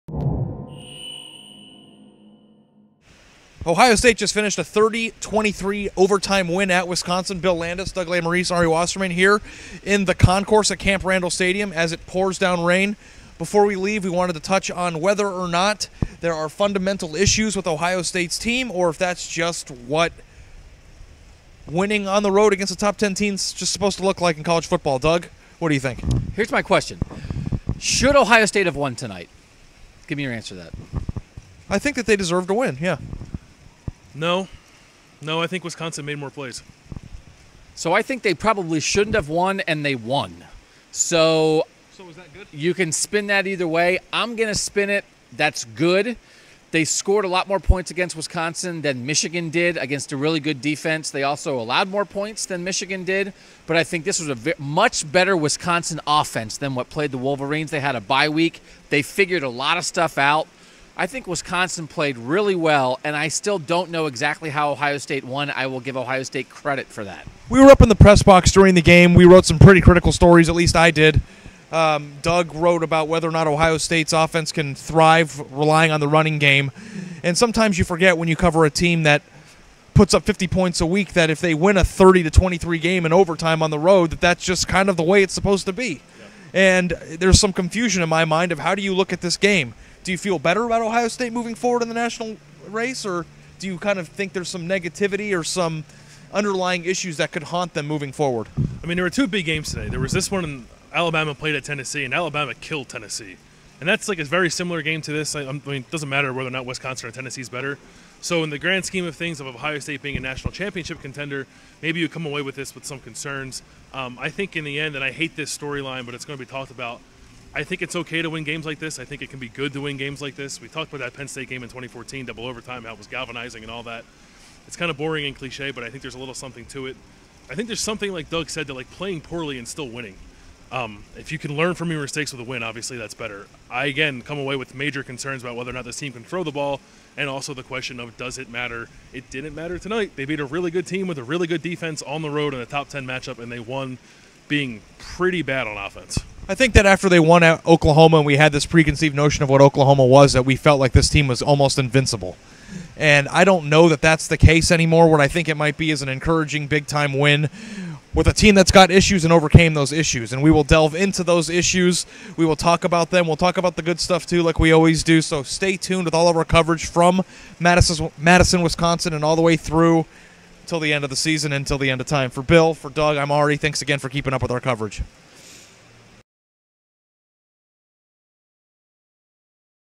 Ohio State beat Wisconsin 30-23 in overtime, what did it mean?
On the No. 2 Buckeyes beating the Badgers.